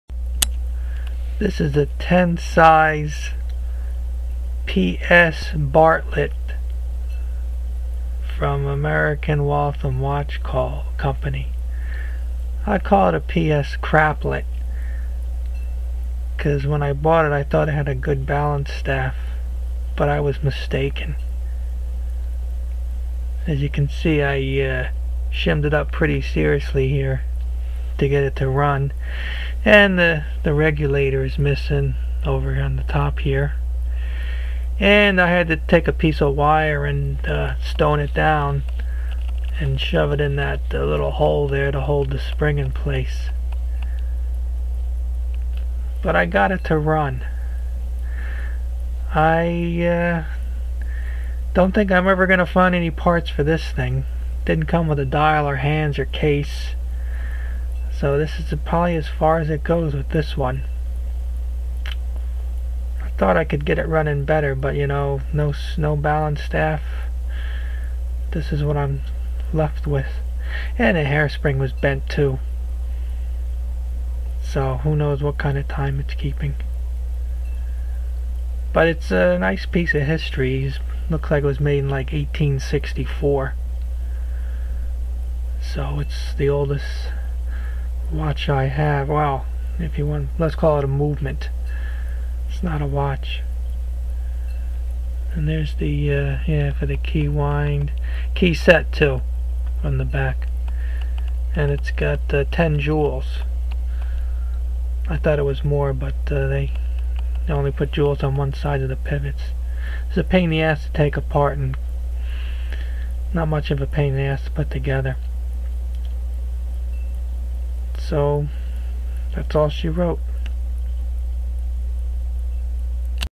P. S. Bartlett pocket watch movement, model 1861, made by American Waltham in 1864.
P. S. Bartlett pocket watch movement, model 1861, made by American Waltham in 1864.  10 size, key wind, key set, 10 jewels, solid balance.